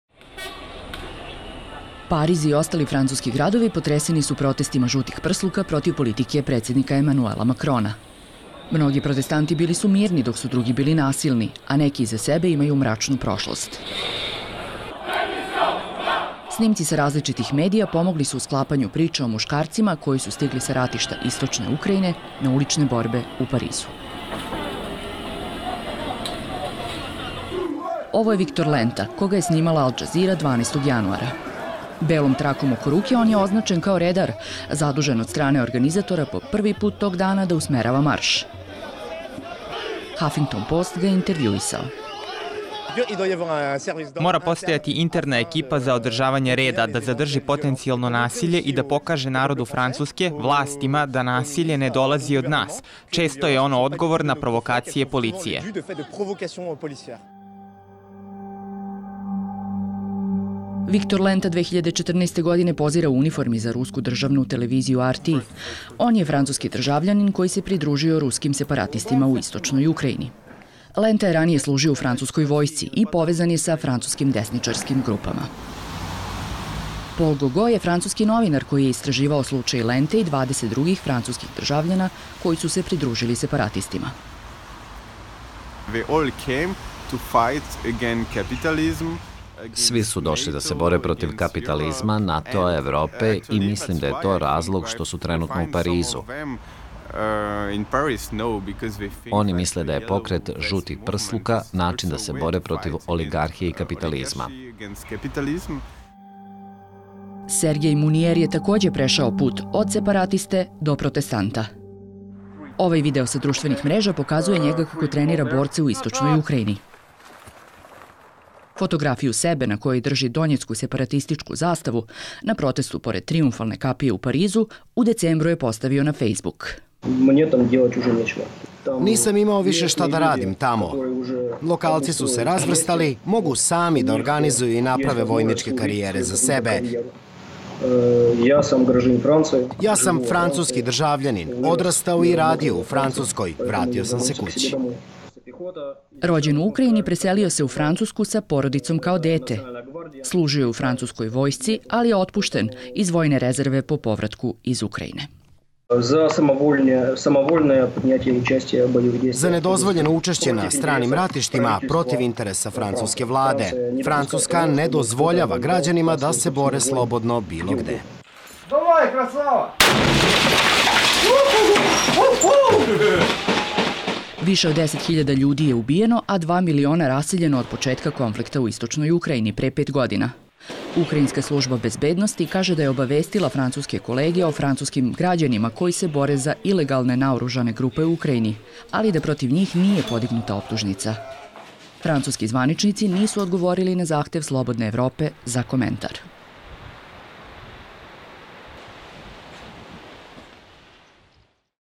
Borci iz Donbasa među 'žutim prslucima'
Francuski građani koji podržalu ruske separatiste u istočnoj Ukrajini, sada su među protestantima 'žutim prslucima' u Parizu, Francuskoj.